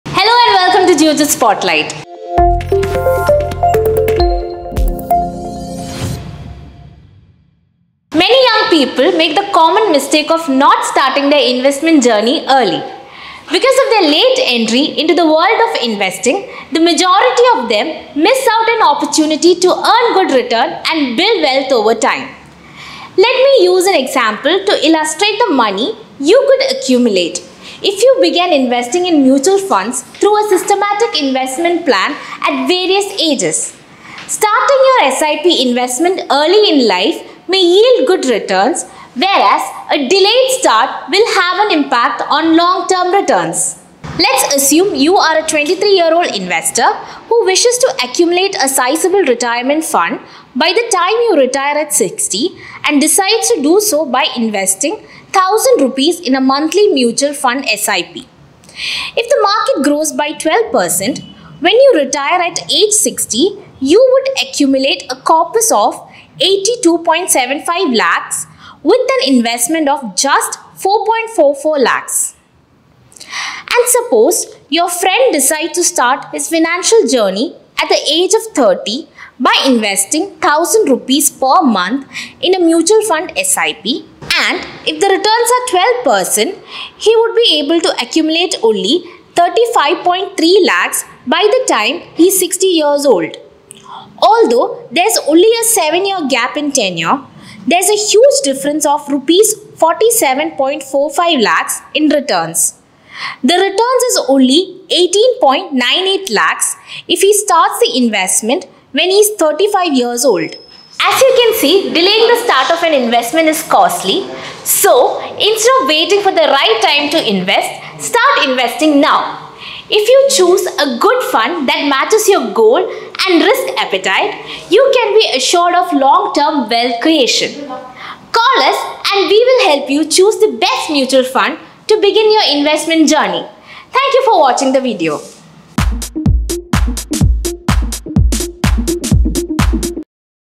Delaying investments can cost you dearly | Best SIPs | SIP Investment
SIP is an easy way to invest in the stock market. If you pay attention, you can get huge benefits through SIP

Geojit Financial Services Ltd
AMFI - registered Mutual Fund Distributor, ARN:0098